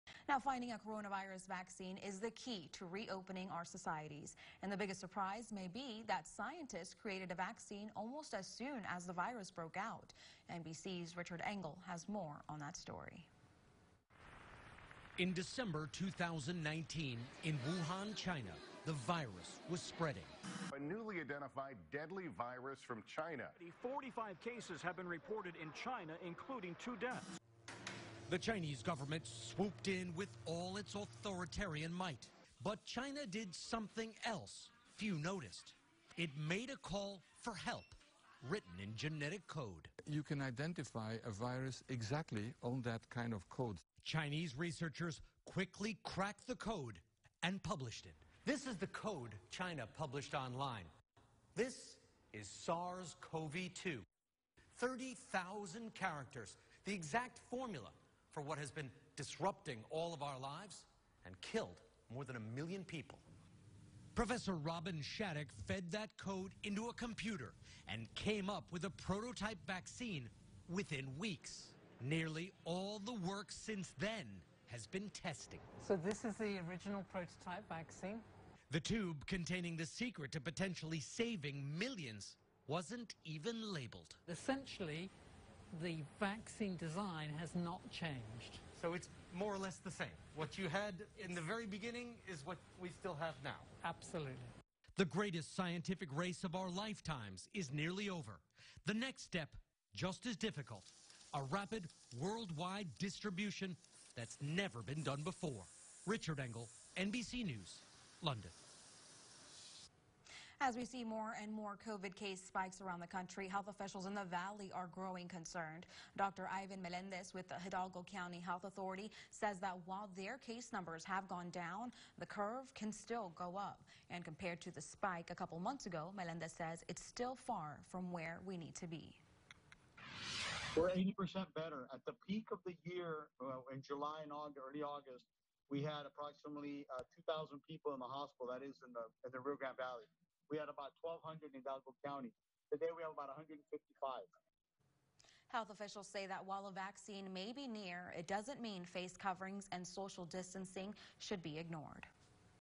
In China, scientist were developing a Coronavirus VACCINE months ago, NBC Special
In December 2019, in Wuhan China the Coronavirus was spreading. 🧫🦠🧬
But what many people didn't know, was that China was already writing the genetic code to formulate a vaccine. 🧪